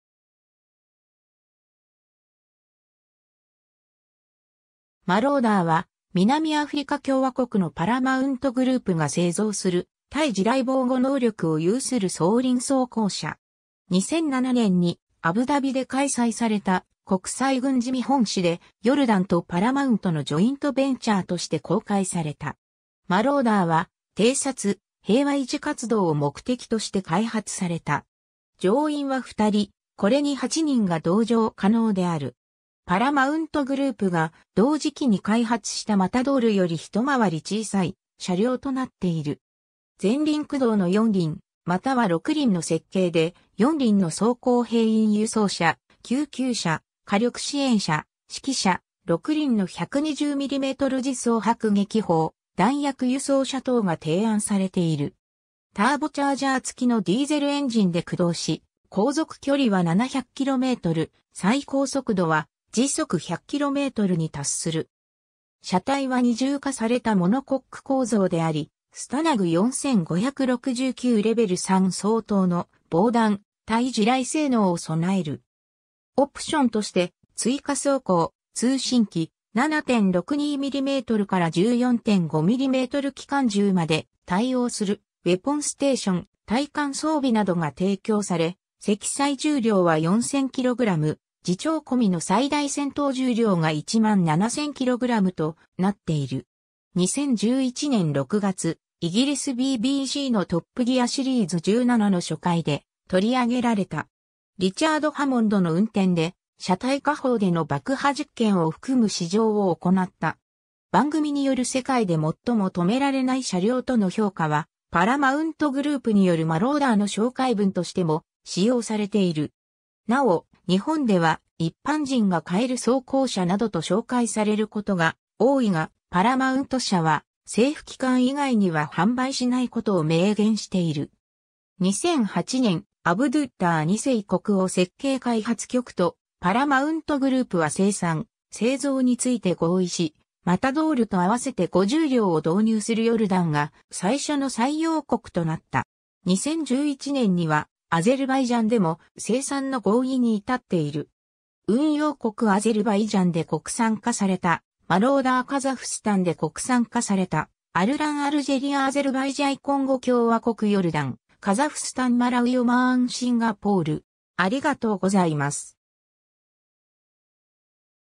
マローダー (装輪装甲車)
マローダー（Marauder）は、南アフリカ共和国のパラマウント・グループが製造する対地雷防護能力を有する装輪装甲車。
2007年にアブダビで開催された国際軍事見本市（IDEX、中東最大の軍事見本市）で、ヨルダンとパラマウントのジョイントベンチャーとして公開された。
 マローダーは偵察・平和維持活動を目的として開発された。
乗員は2人、これに8人が同乗可能である。
パラマウント・グループが同時期に開発したマタドールより一回り小さい車両となっている。
 全輪駆動の4輪または6輪の設計で、4輪の装甲兵員輸送車、救急車、火力支援車（対戦車、対人）、指揮車、6輪の120mm自走迫撃砲、弾薬輸送車等が提案されている。
ターボチャージャー付きのディーゼルエンジンで駆動し、航続距離は700km、最高速度は100km/hに達する。
 車体は二重化されたモノコック構造であり、STANAG 4569レベル3相当の防弾・耐地雷性能を備える。
 オプションとして追加装甲、通信機、7.62mmから14.5mm機関銃まで対応するウェポンステーション、耐寒装備などが提供され、積載重量は4,000kg、自重込みの最大戦闘重量が17,000kgとなっている。
 2011年6月、イギリスBBCのトップ・ギアシリーズ17の初回で取り上げられた。
リチャード・ハモンドの運転で車体下方での爆破実験を含む試乗を行った。
番組による世界で最も止められない車両（world's most unstoppable vehicle）との評価は、パラマウント・グループによるマローダーの紹介文としても使用されている。
 なお、日本では「一般人が買える装甲車」などと紹介される事が多いが、パラマウント社は政府機関以外には販売しないことを明言している。
 2008年、アブドゥッラー2世国王設計開発局とパラマウント・グループは生産・製造について合意し、マタドールと合わせて50両を導入するヨルダンが最初の採用国となった。
2011年には、アゼルバイジャンでも生産の合意に至っている。
 運用国（2015年） アゼルバイジャンで国産化されたマローダー（2011年、バクー） カザフスタンで国産化されたアルラン アルジェリア アゼルバイジャン コンゴ共和国 ヨルダン カザフスタン マラウイ オマーン シンガポール